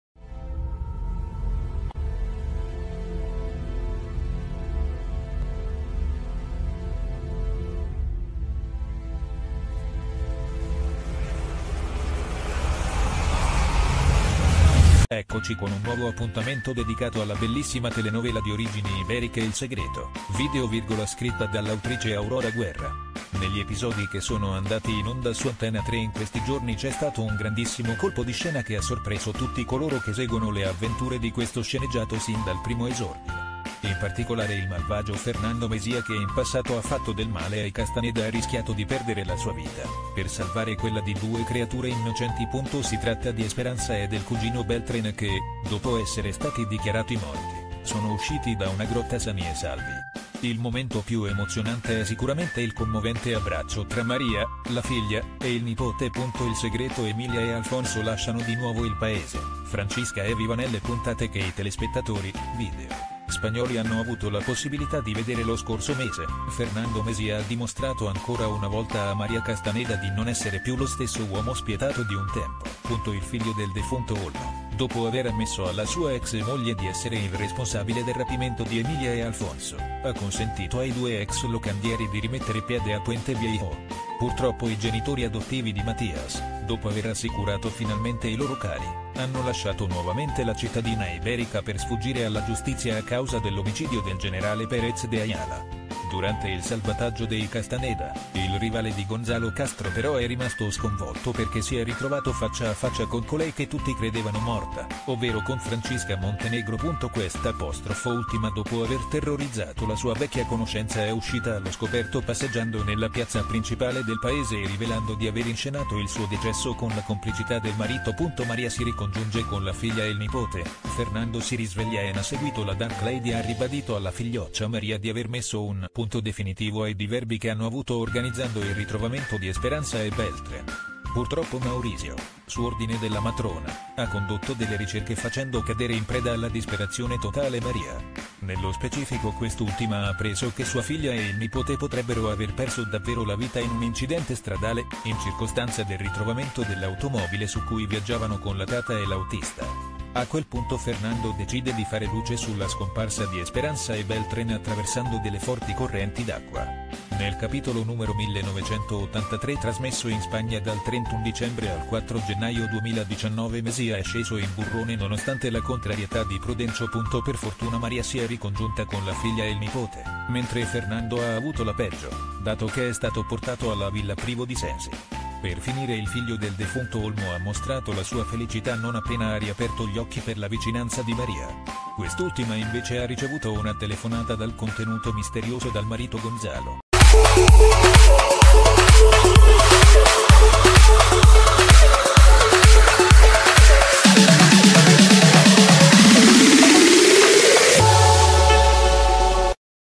Anticipazioni spagnole Il Segreto: Maria riabbraccia Esperanza e Beltran grazie a Fernando
Notizie calde: Anticipazioni spagnole Il Segreto: Maria riabbraccia Esperanza e Beltran grazie a Fernando